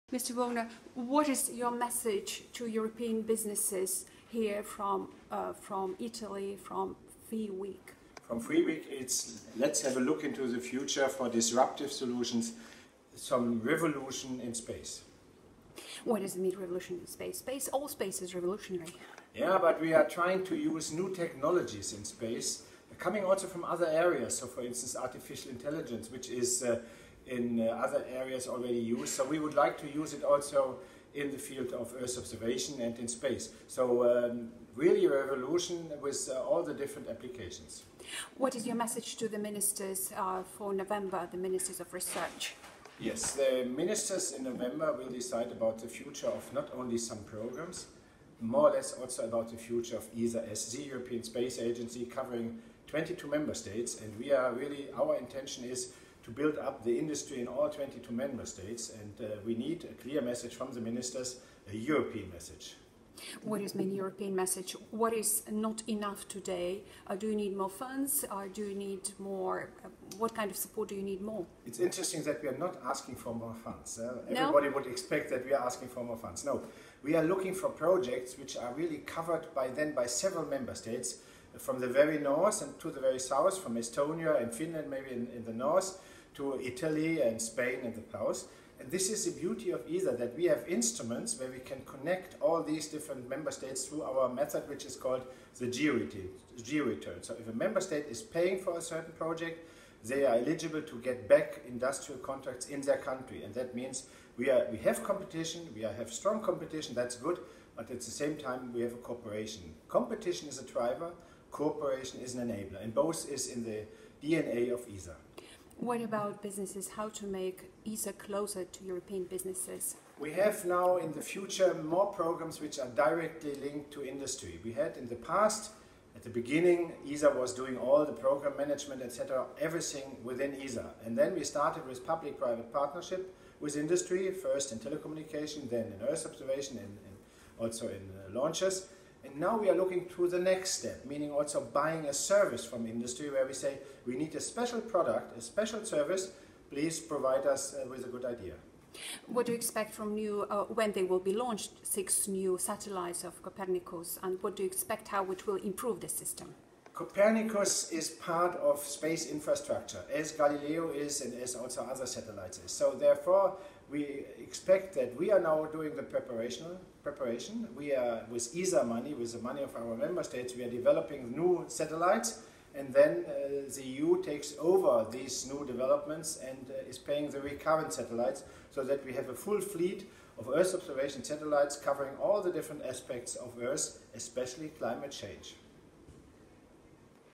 ESA Woerner calls for revolution
ESA Director General shares his views on benefits of Earth Observation to economies and communities.